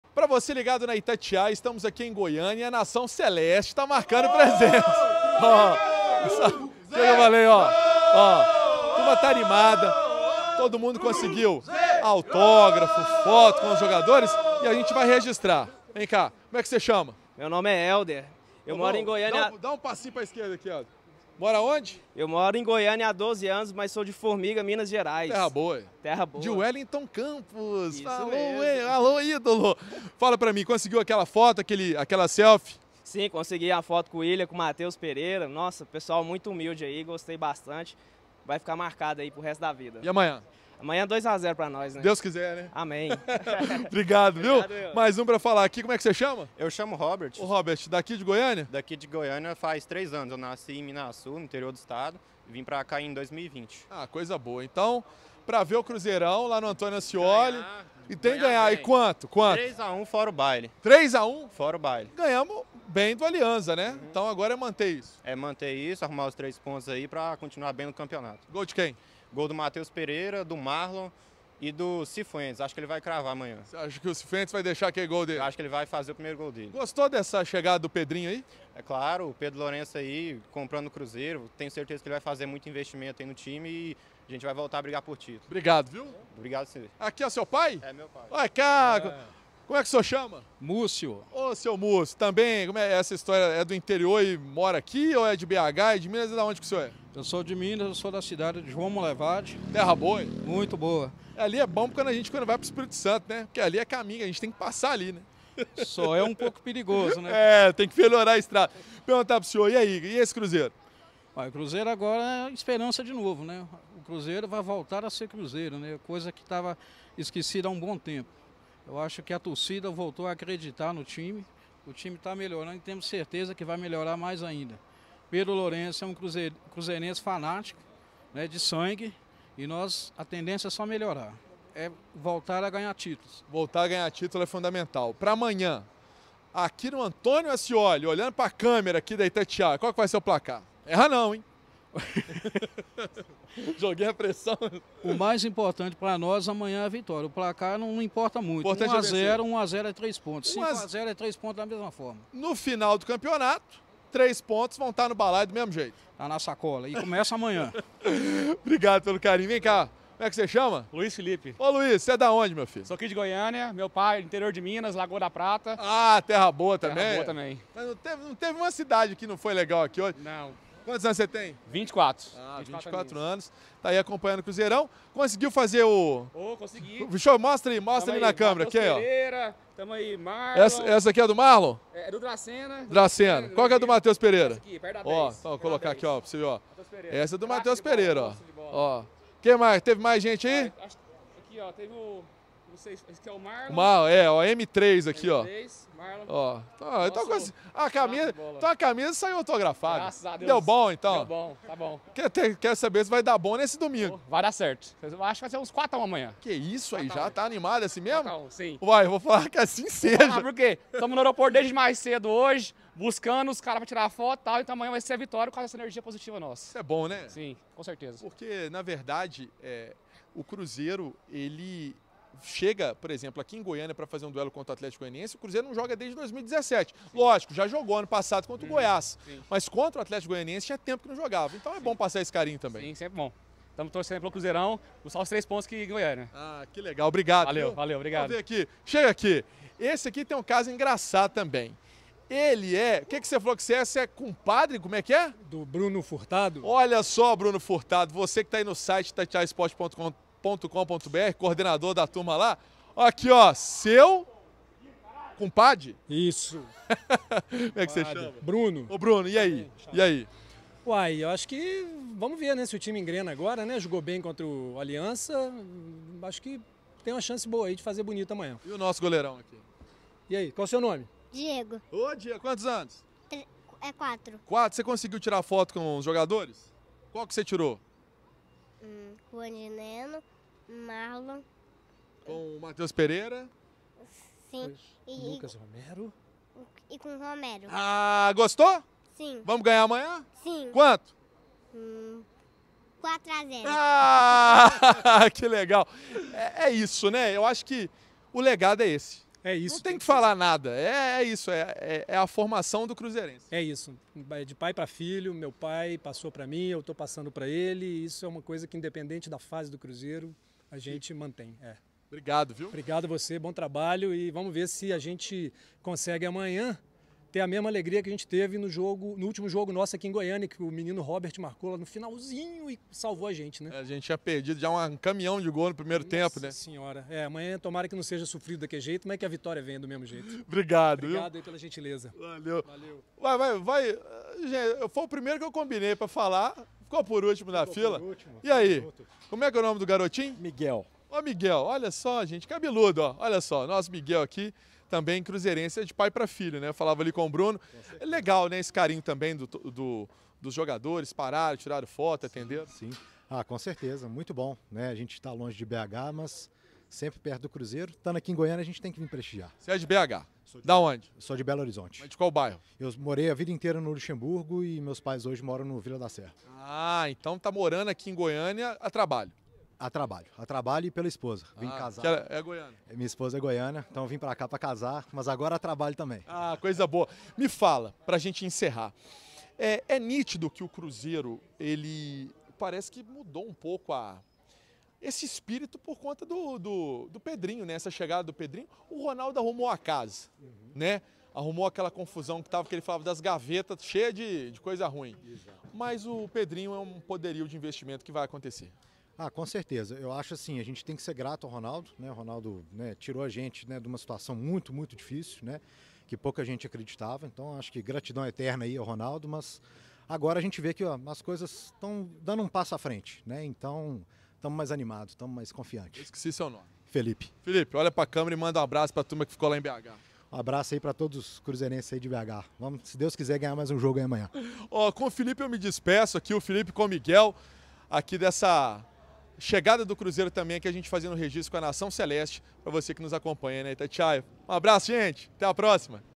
TORCIDA DO CRUZEIRO MARCA PRESENÇA EM GOIÂNIA PARA RECEBER EQUIPE DO CRUZEIRO!
O repórter Emerson Pancieri acompanhou a chegada do time do Cruzeiro em Goiânia